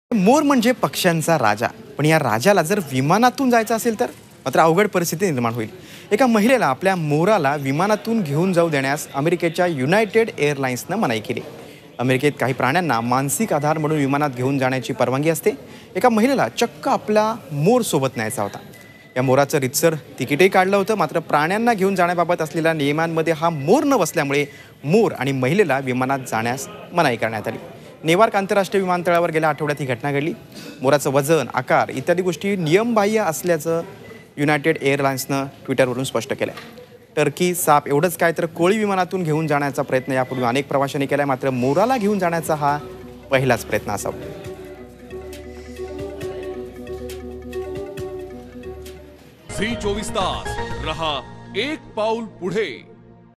अमेरिका | प्रशासनाच्या विरोधामुळे तिकीट काढलेल्या मोराचा विमानप्रवास बारगळला
Americas United Airline Denied Transportation Of Peacock From Airline With Passenger